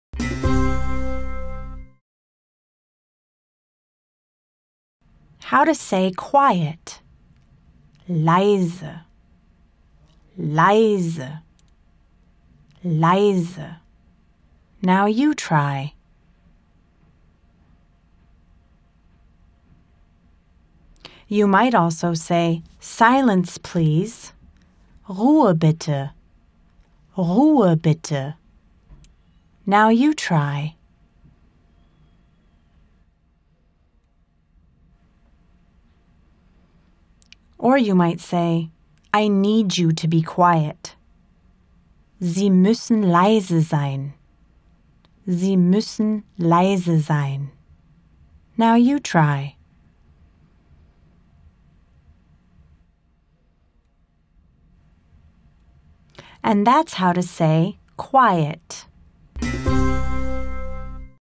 How to Say "Quiet" in German | German Lessons
Like these German Lessons !!!

Learning how to speak German? In this video, you'll learn how to say "quiet" in German.

How to say, "Quiet." Leise. Leise.
Now you try.
You might also say, "Silence, please." Ruhe bitte. Ruhe bitte.
Now you try.
Or you might say, "I need you to be quiet." Sie mussen leise sein. Sie mussen leise sein.
Now you try. And that's how to say, "Quiet."